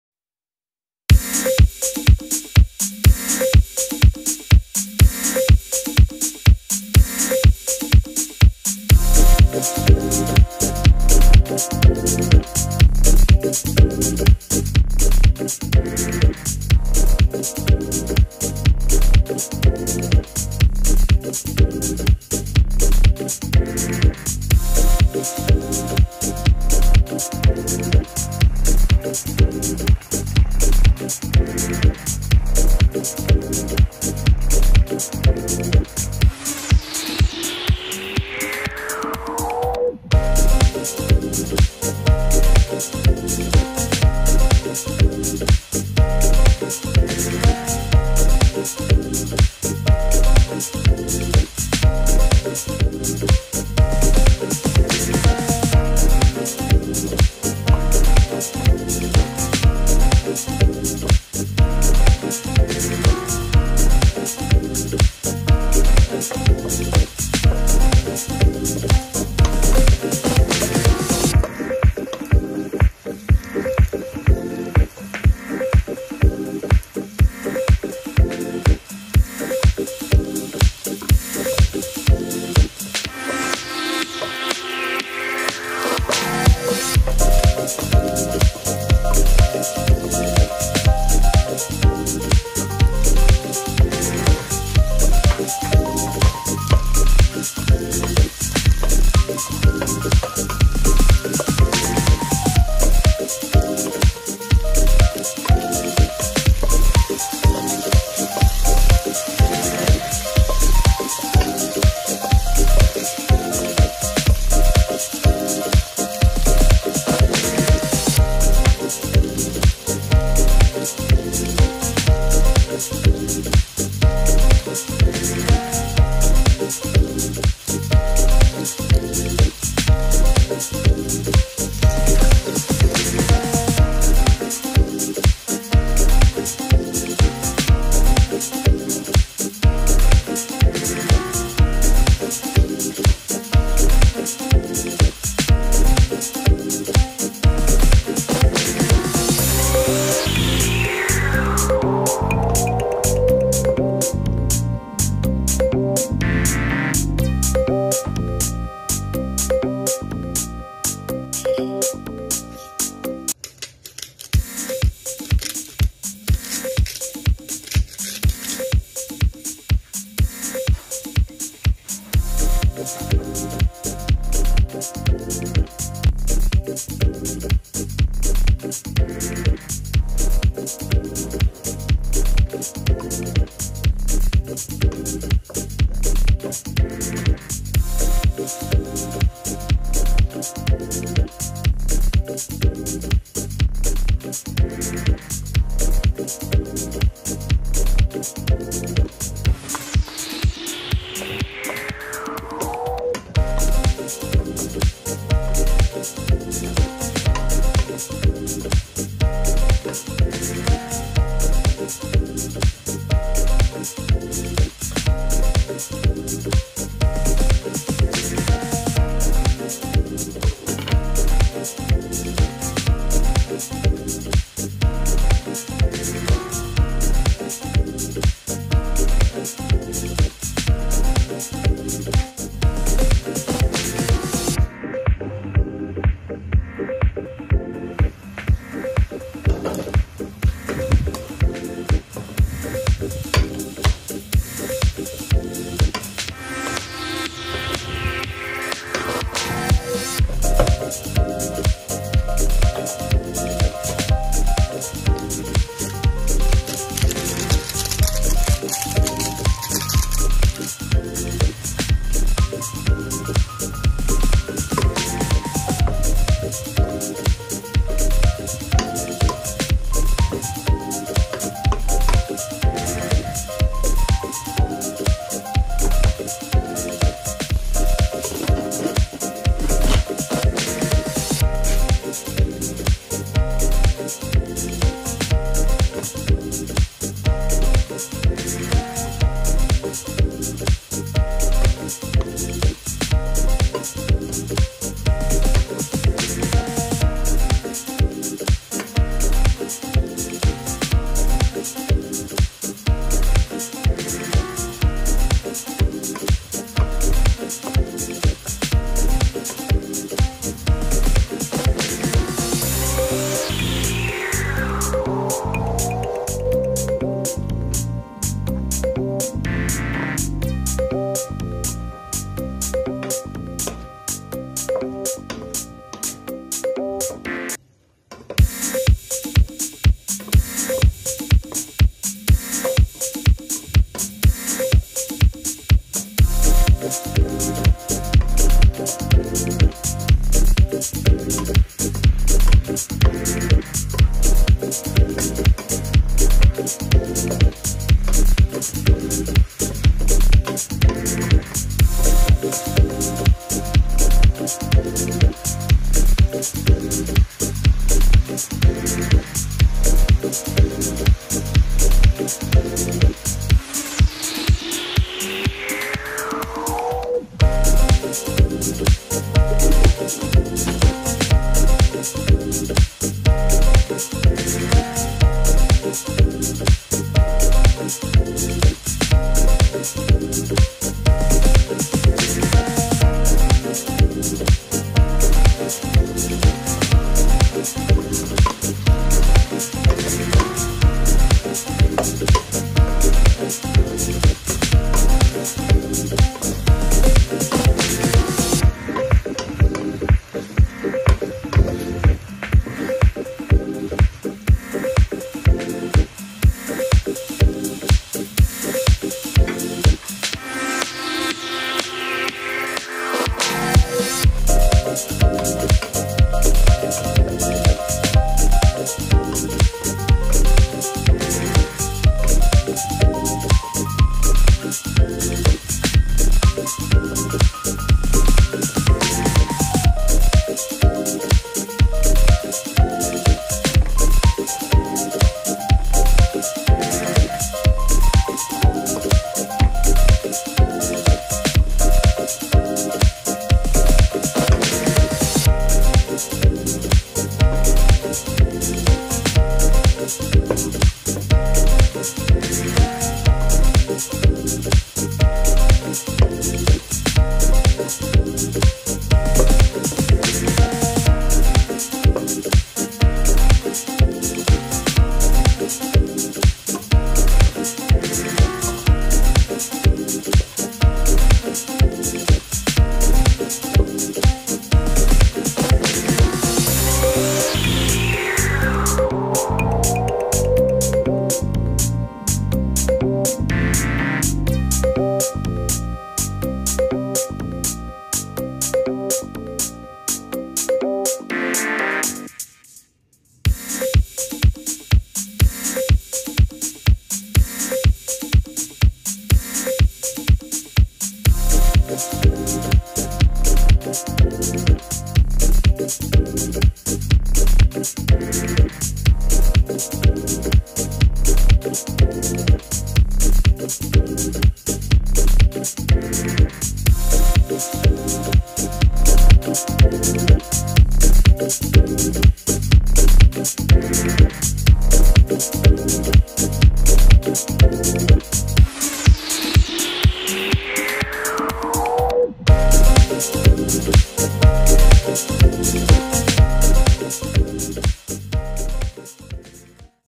PASTA w/CAULIFLOWER & POTATO Panlasang Pinoy
Ingredients:
*Penne pasta
*Cauliflower
*Potato
*Smoked Sausage
*cooking oil
*Salt & Pepper

Pls Like & Subscibe

See you again on my next vlog! keepsafe & God bless..